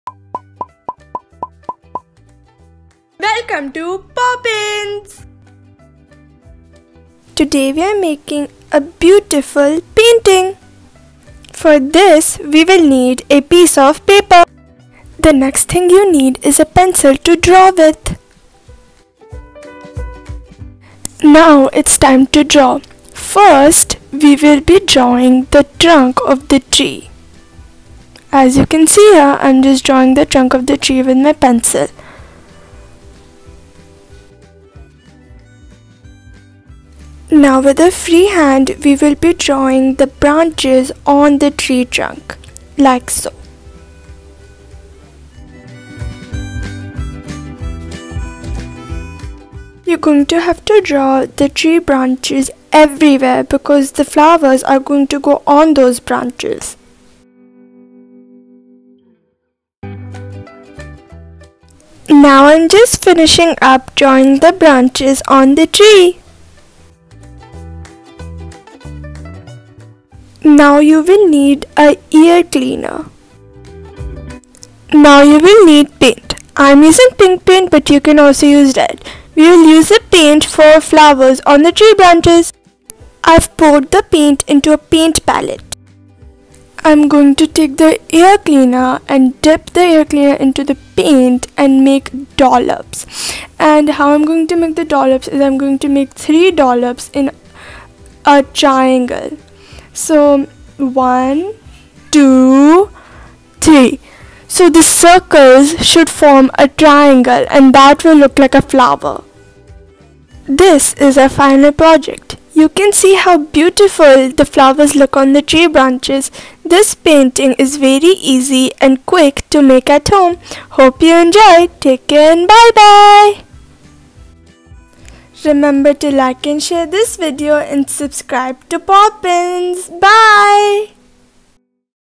Easy Cherry blossom Painting for kids | Fun Painting for kids | Fun learning
Quick and Easy Cheer blossom painting with ear cleaner for kids. Kids will love to make and enjoy this easy activity.